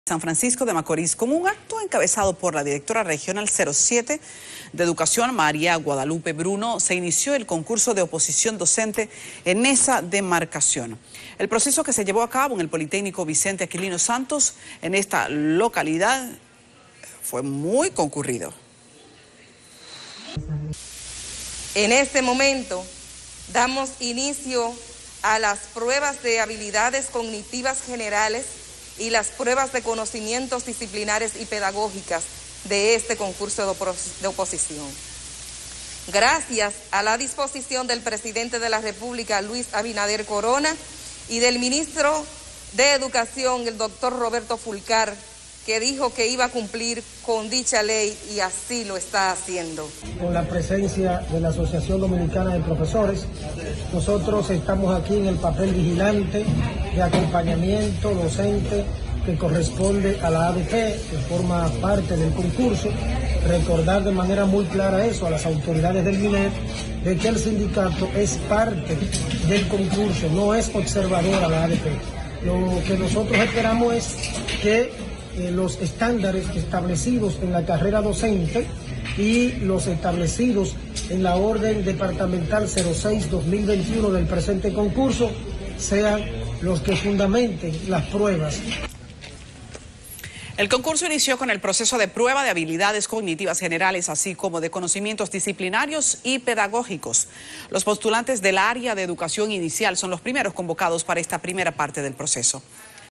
Inician proceso de prueba de habilidades cognitivas generales para maestros en SFM
En San Francisco de Macorís, con un acto encabezado por la directora regional 07 del Ministerio de Educación, María Guadalupe Bruno, se inició el Concurso de Oposición Docentes en esta demarcación.